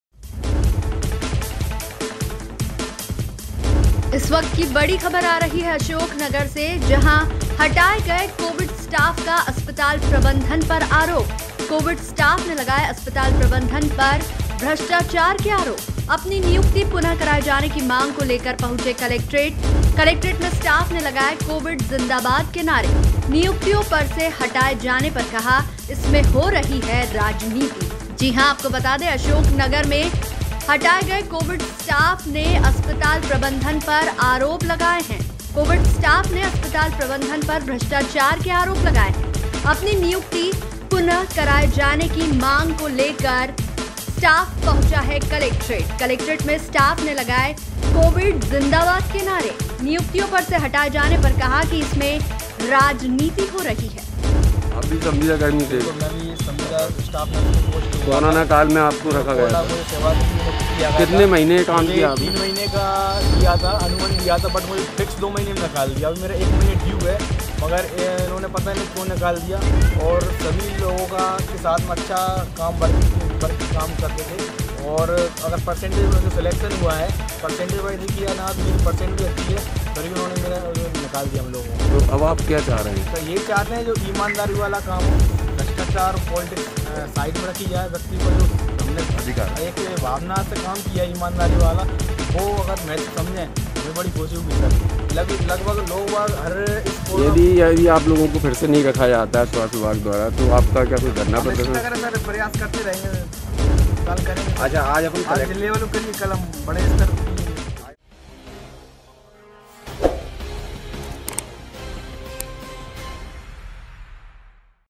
Breaking | Ashoknagar - हटाए गए covid स्टाफ का अस्पताल प्रबंधन पर आरोप
कोविड स्टाफ का अस्पताल प्रबंधन पर आरोप
कोविड स्टाफ ने लगाए अस्पताल प्रबंधन पर लगाएं भ्रष्टाचार के आरोप
अपनी नियुक्ति पुनः कराए जाने की मांग लेकर पहुंचे कलेक्ट्रेट
कलेक्ट्रेट में स्टाफ ने लगाए कोविड जिंदाबाद के नारे